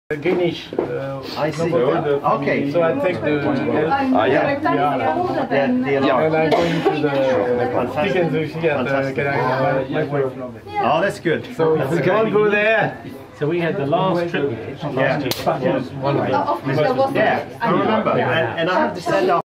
Champagne H.Blin Magnum 2004 opening
London tasting January 2018 - Six Portland Street Holland Park. Champagne H.Blin Magnum Vintage 2004 opening.